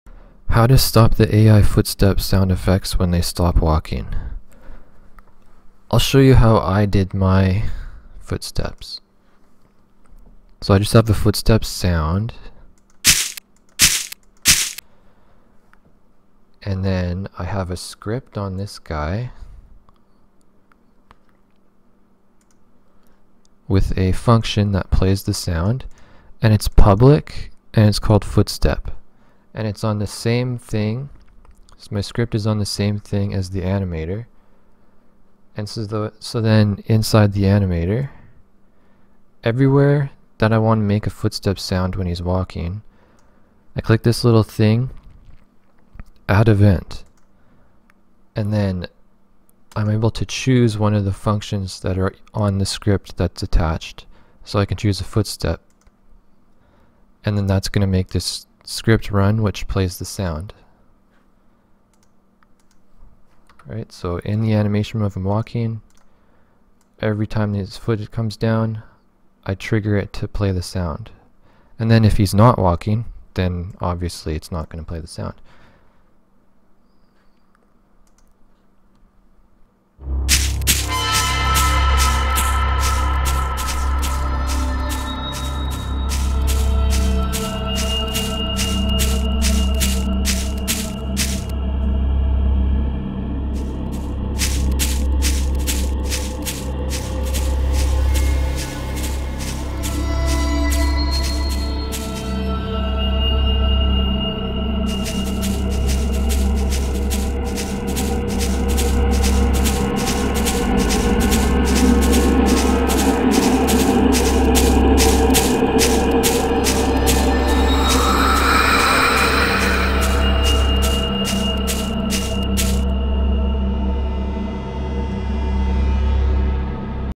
Enemy Footstep Sounds
How to trigger footstep sounds from events in the walk animation.